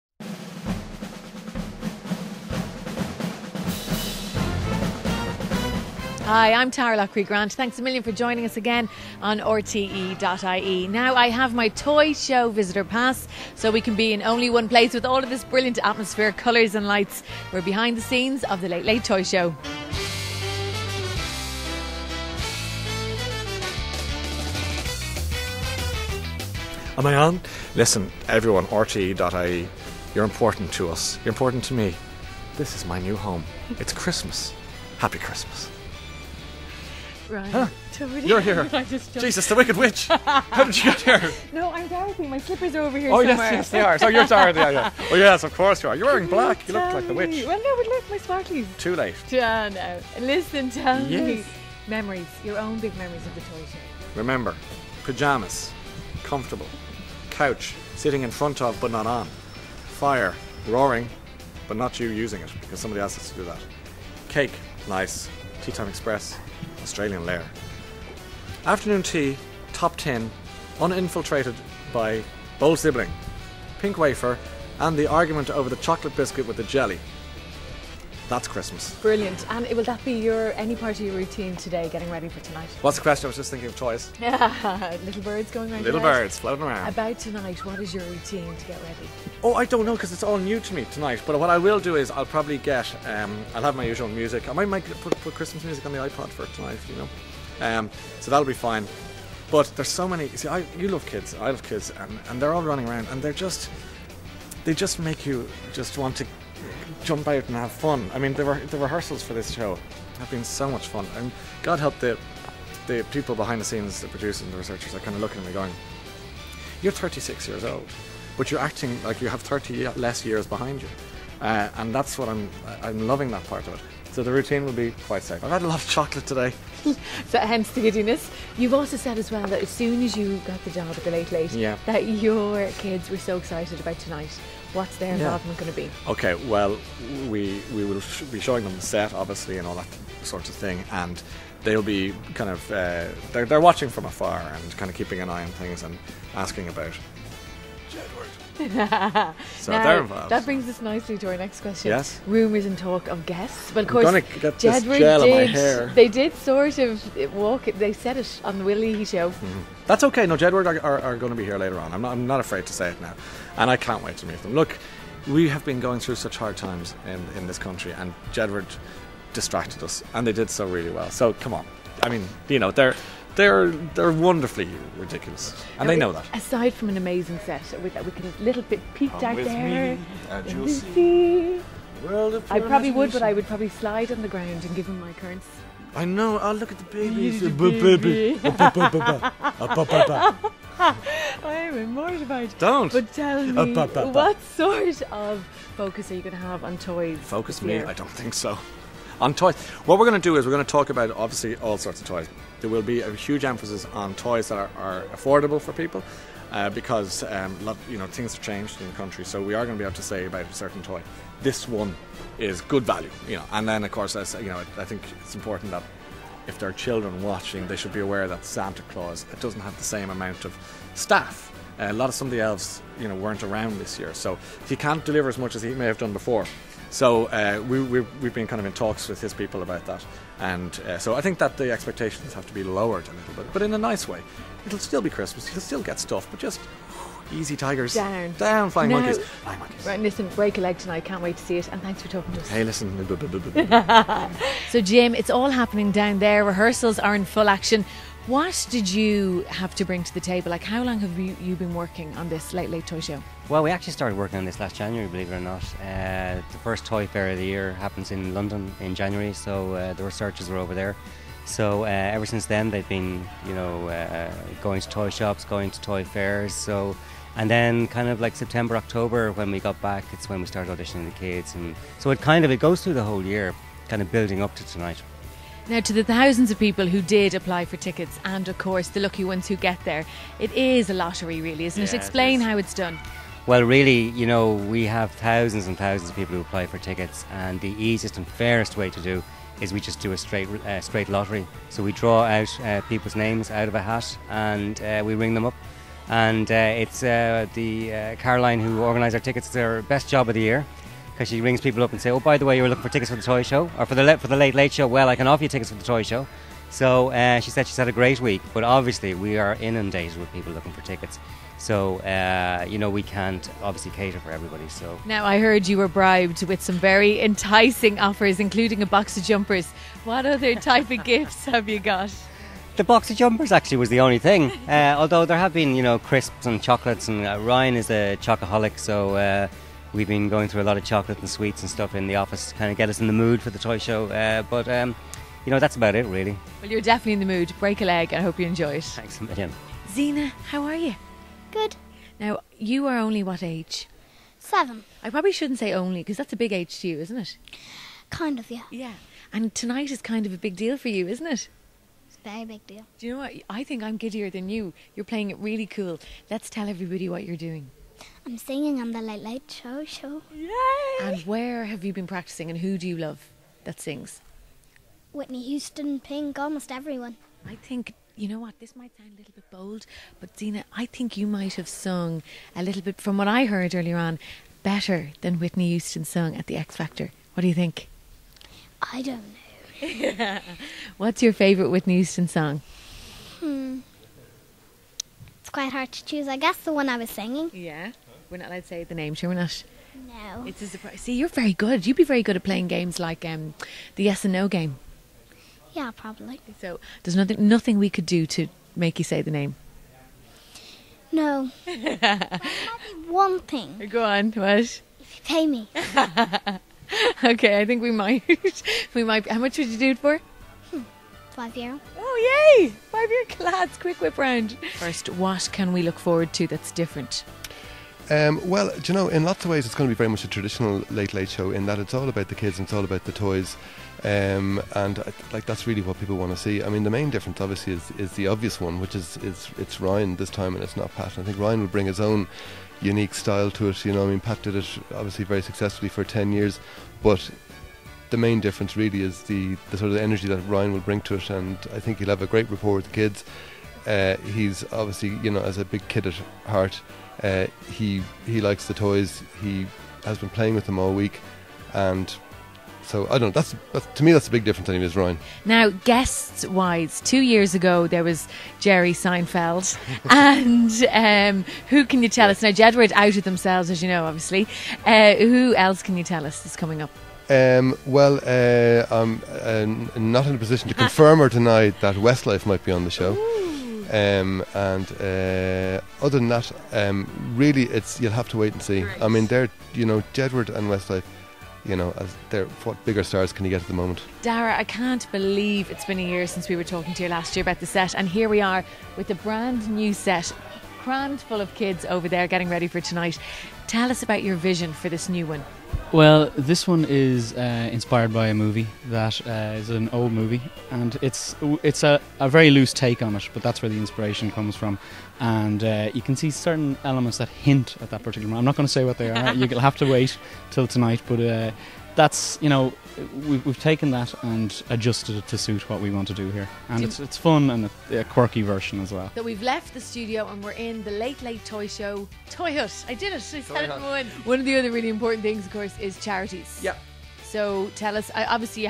Behind the scenes at the Late Late Toy Show
The Late Late Toy Show: Ryan Tubridy talks to RTÉ.ie's Taragh Loughrey-Grant.